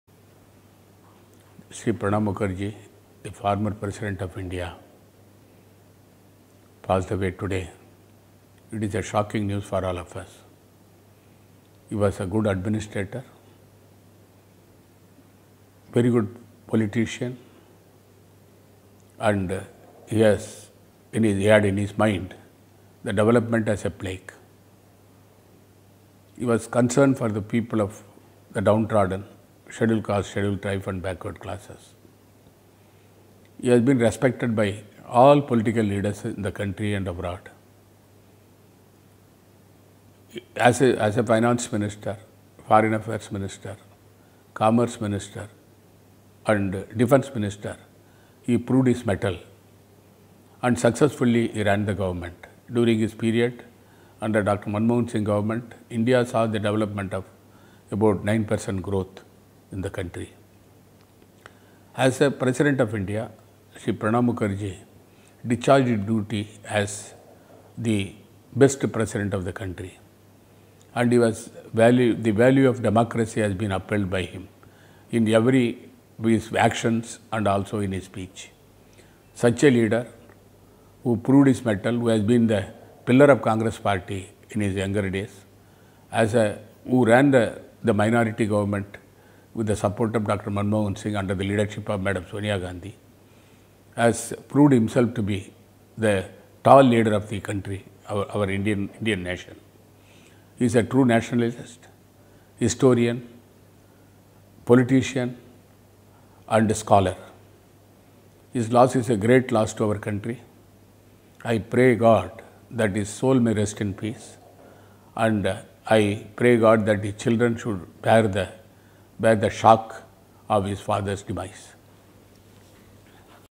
We grieve the loss of a dynamic leader, former President of India, Bharat Ratna Shri PranabMukherjee
We grieve the loss of a dynamic leader, the former President of India, Bharat Ratna Shri Pranab Mukherjee. Deepest condolences to his family. May his soul rest in peace.